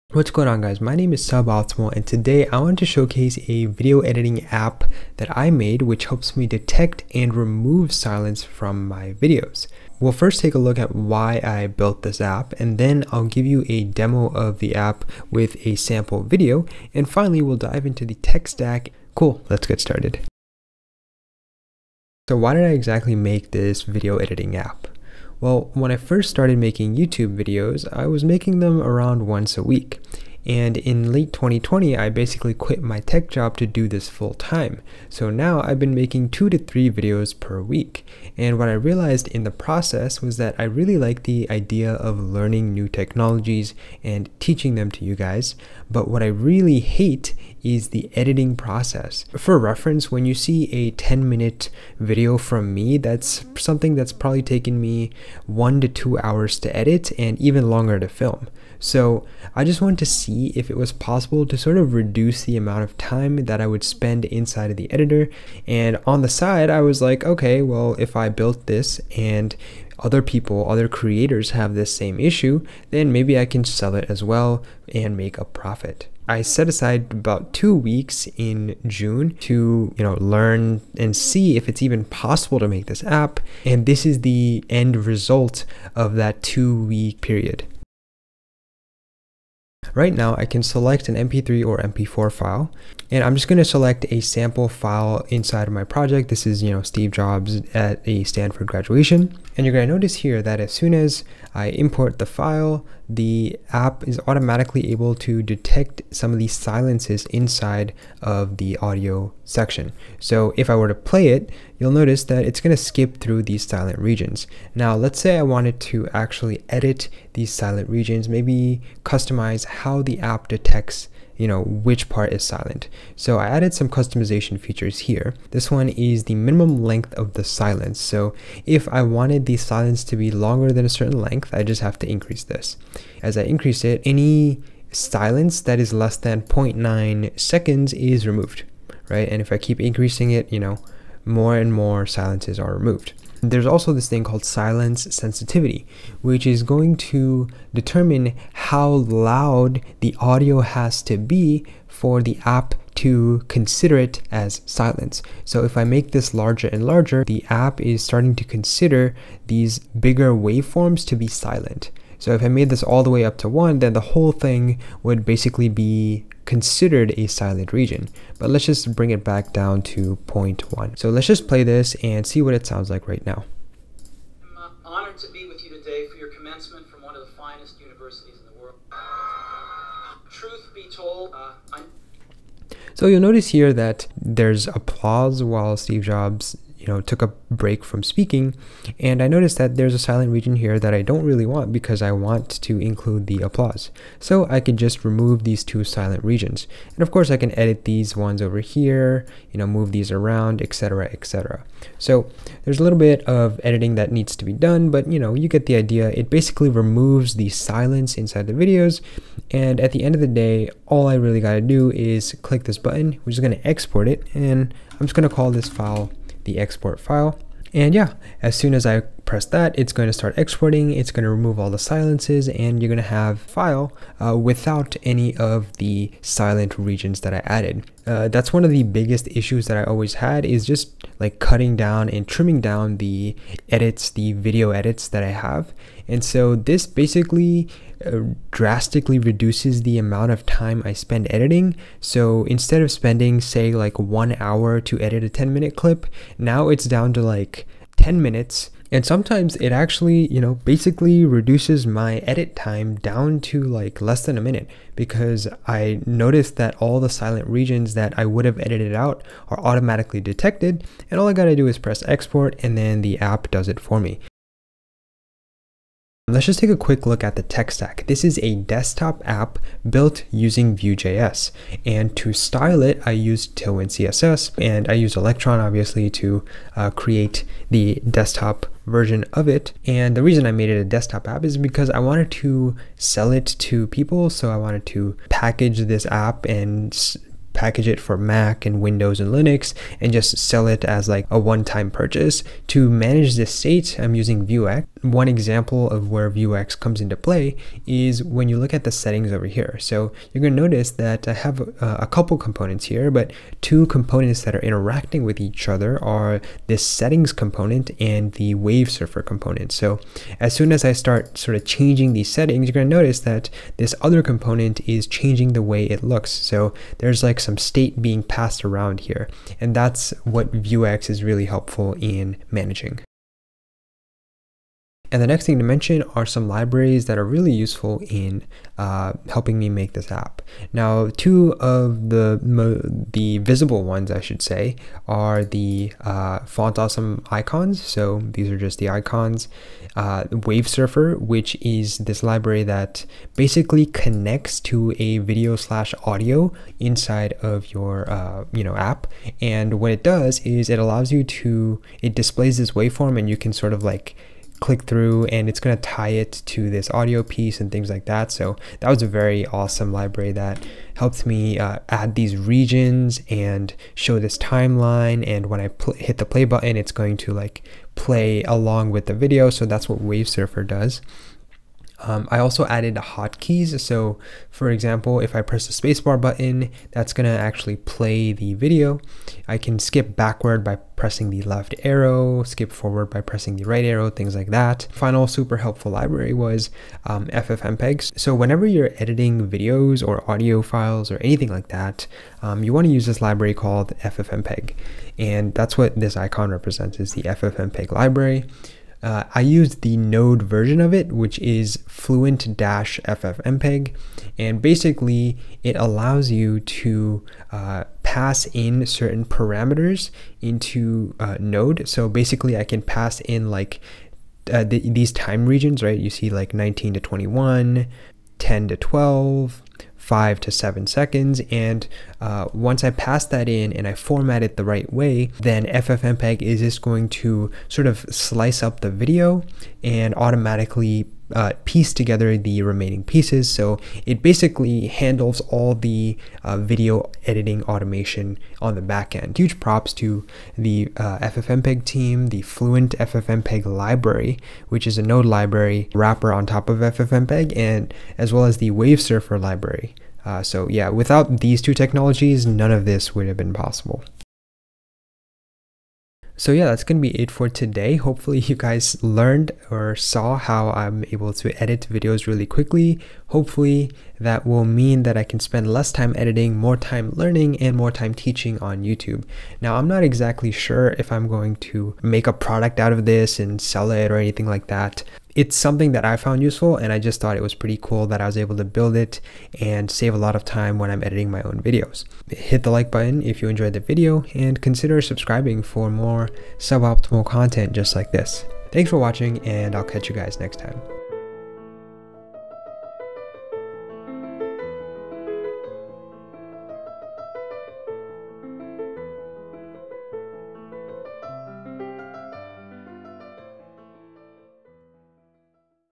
I Made an App that Automatically Edits Videos with Vue and Electron JS
In 2020, I quit my tech job to pursue creating content on YouTube full-time. The biggest problem I had with making videos was editing/removing silence from them. For reference, it takes 1 hour to edit 10 minutes of video. The engineer in me was itching to solve this problem so I put aside 2 weeks in June to learn video editing technologies (WaveSurfer.js + FFmpeg) and make a proof of concept - this is the result. It's really rough around the edges but saves me a lot of time while editing.

Main Features:
- Automatically detect silence in the video
- Regenerate silent regions based on length and loudness
- Remove detected silent regions on export
- Add/remove padding to silent regions

Tech Stack:
- Vue/Vuex
- Electron.js (desktop app)
- Tailwind CSS (core UI)
- WaveSurfer.js (audio wave display)
- Fluent FFmpeg (export edited video)
- Font Awesome (icons)
- Hotkeys (keyboard shortcuts)

== [ Timestamps ] ==
00:00 Intro
00:20 Why Make an App?
01:28 Demo
04:24 Tech Stack
05:23 Helpful JS Libraries
07:34 Fin